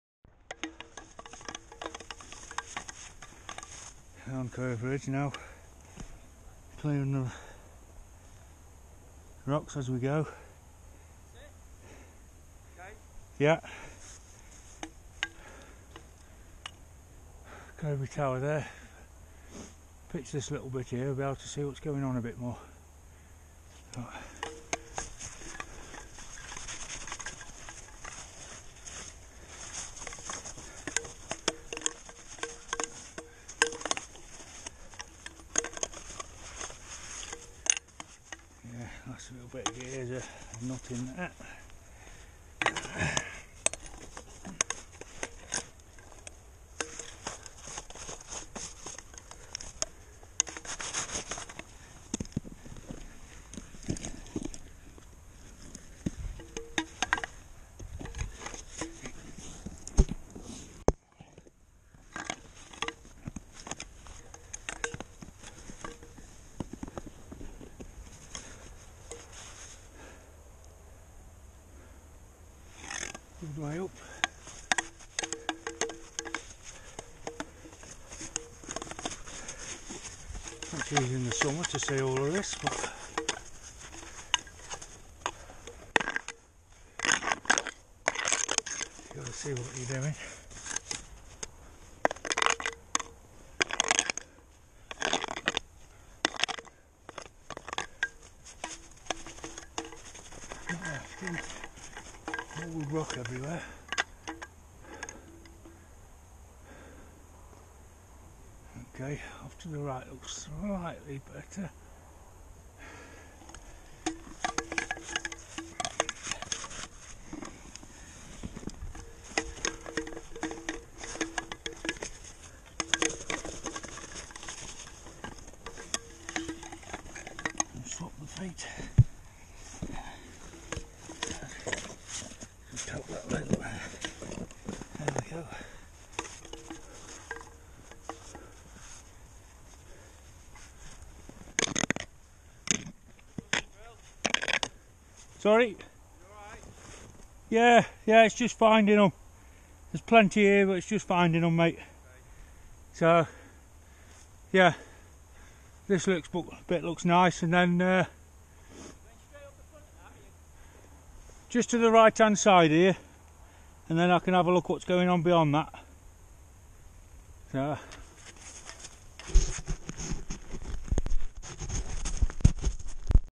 Buachaille Etive Mor Curved Ridge 22 01 2019 Clearing snow finding placements
Team (John, Matt Rich, Brian and I)
A 36 hour weather window opportunity, Mixed ground proper, Fresh snow and rotten ice.
On the first technical pitch of Curved Ridge. Clearing snow to find hand/foot and gear placements. Slow going, but plenty of good holds hiding in the snow.

Note:
Make sure you have all required equipment on your adventure.

Ambassadors: Eagle360sport
Technical, Casual, Sport Apparel & Equipment